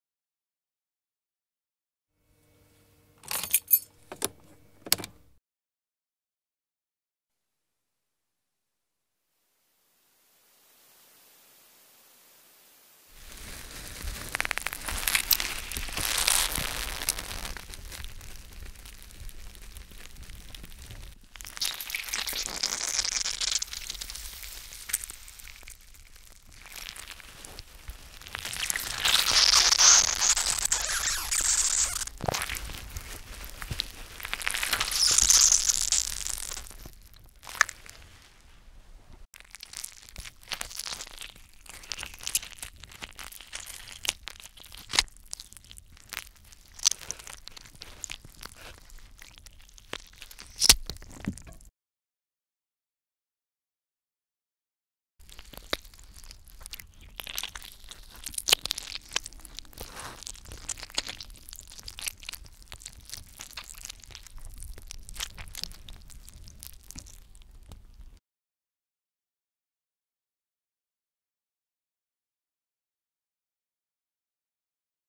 Wsłuchaj się: Cytryna
Dźwięk wyciskanej i "maltretowanej" cytryny, zarejestrowany w komorze bezechowej LAT KMiW AGH w Krakowie przy użyciu ekstremalnie czułego mikrofonu.

0:00 – intro
0:12 – wyciskanie cytryny
0:38 – wydłubywanie miąższu